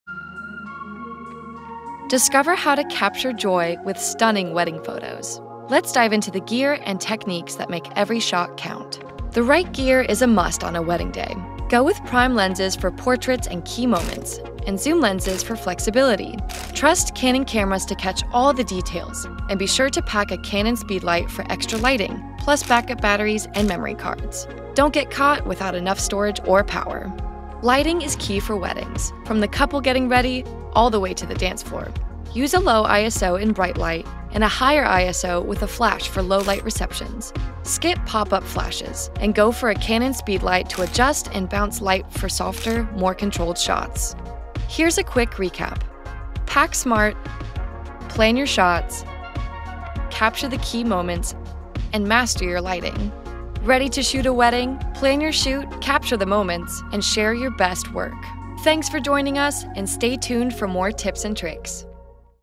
Capture Weddings: Essential Tips with Canon
Weddings are all about capturing emotion and joy. Discover essential tips and tricks to ensure every shot tells a beautiful story. We’re here to help you capture every moment with creativity and precision.